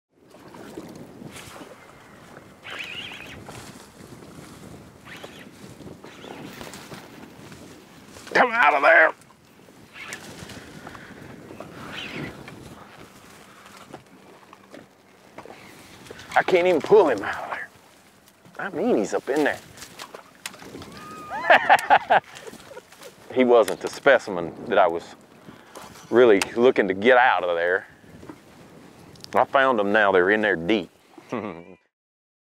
Hack found 'em in That THICK Cover
Greg Hackney is known for fishing heavy cover on the regular and in and upcoming Wired2fish video, he found 'em thick in the reeds where it's not always easy to get them to the boat!